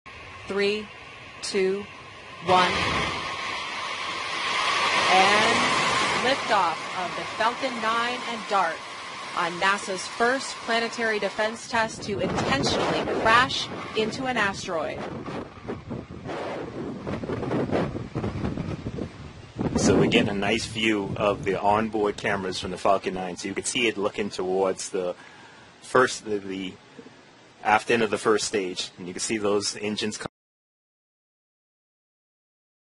Asteroidlerin yörüngesini değiştirmek için geliştirilen uzay aracı fırlatıldı
NASA tarafından Dünya’ya çarpma riski bulunan asteroidleri itme yöntemiyle yörüngelerini değiştirmek için geliştirilen uzay aracı başarıyla fırlatıldı.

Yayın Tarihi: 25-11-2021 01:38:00